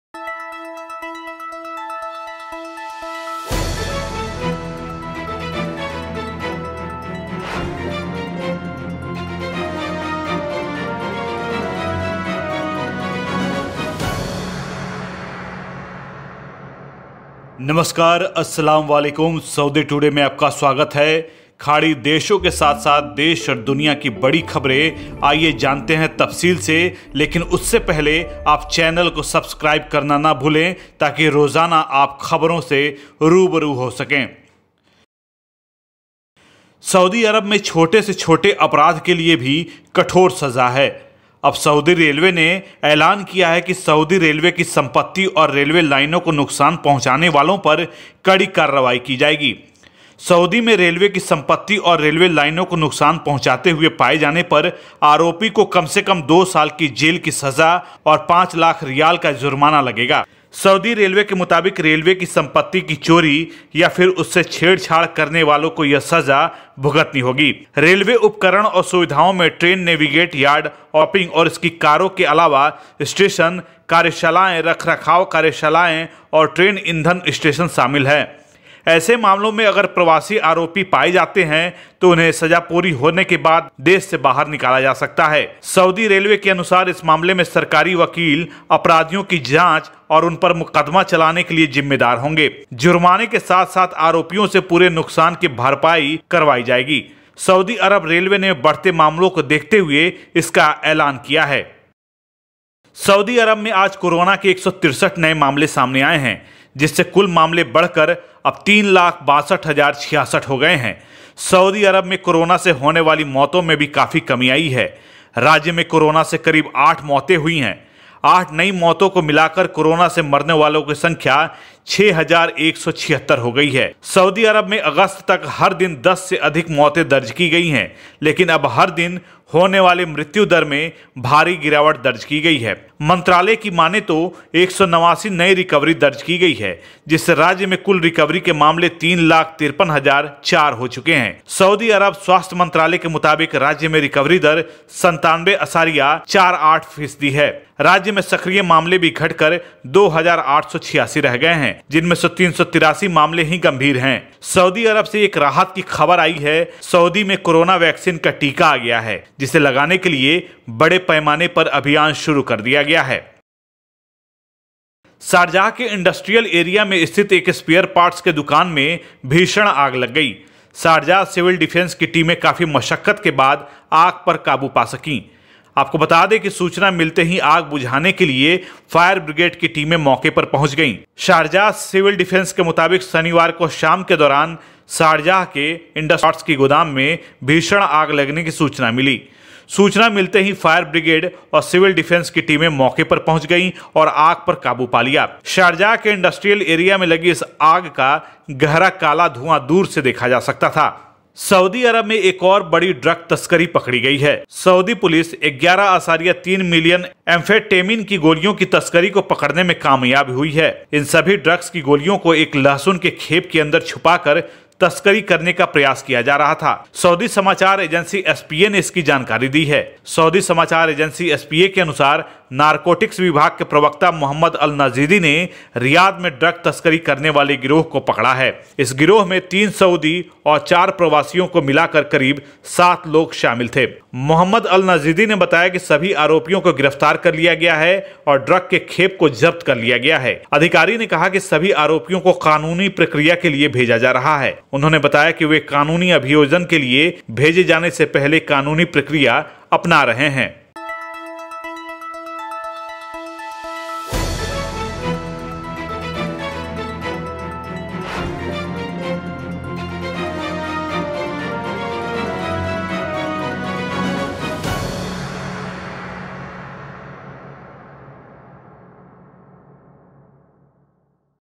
Saudi Arabia made Strict Law । Saudi Arabia News Today and updates
1. saudiarabia में छोटे से छोटे अपराध के लिए भी कठोर सजा है। अब saudirailway ने ऐलान किया है कि सऊदी रेलवे की सम्पत्ति और railwaylines को नुकसान पहुंचाने वालों पर कड़ी कार्रवाई की जायेगी। सऊदी में रेलवे की सम्पत्ति और रेलवे लाइनों को नुकसान पहुंचाते हुए पाये जाने पर आरोपी को कम से कम 2 साल की जेल की सजा और 5 लाख riyal का fine लगेगा।

2. saudiarabia में आज corona के 163 नए मामले सामने आये हैं। जिससे कुल मामले बढ़कर अब 3 लाख 62 हजार 066 हो गए हैं। saudi arab में covid-19 से होने वाली deathtoll में भी काफी कमी आई है। राज्य में कोरोना से करीब 8 मौतें भी हुई हैं। 8 नई मौतों को मिलाकर covid-19 से मरने वालों की संख्या 6,176 हो गयी है। saudiarabia में अगस्त तक हर दिन 10 से अधिक मौतें दर्ज की गई हैं लेकिन अब हर दिन होने वाली मृत्यु दर में भारी गिरावट दर्ज की गई है।

3. sharjah के इंडस्ट्रियल एरिया में स्थित एक spareparts के गोदाम में भीषण fire लग गई। sharjahcivildefence की टीमें काफी मशक्कत के बाद आग पर काबू पा सकी। विदित है कि सूचना मिलते ही आग बुझाने के लिए firebrigade की टीमें मौके पर पहुँच गई। sharjahcivildefence के मुताबिक, शनिवार को शाम के दौरान शारजाह के इंडस्ट्रियल एरिया में स्थित एक स्पेयर पार्ट्स के गोदाम में भीषण आग लगने की सूचना मिली, सूचना मिलते ही फायर ब्रिगेड और सिविल डिफेन्स की टीमें मौके पर पहुँच गई और आग पर काबू पा लिया। । शारजाह के इंडस्ट्रियल एरिया में लगी इस आग का गहरा काला धुंआ दूर से देखा जा सकता था।

4. saudiarabia में एक और बड़ी drugspedaler पकड़ी गई है। saudipolice 11.3 मिलियन एम्फ़ैटेमिन की गोलियों की तस्करी को पकड़ने में कामयाब हुई है। इन सभी ड्रग्स की गोलियों को एक लहसुन के खेप के अंदर छुपा कर तस्करी करने का प्रयास किया जा रहा था। saudinewsagency spa ने इसकी जानकारी दी है। सऊदी समाचार एजेंसी एसपीए के अनुसार, narcoticsdepartment के प्रवक्ता मोहम्मद अल नजीदी ने रियाद में ड्रग तस्करी करने वाले गिरोह को पकड़ा है। इस गिरोह में 3 सऊदी और 4 प्रवासियों को मिला कर करीब 7 लोग शामिल थे।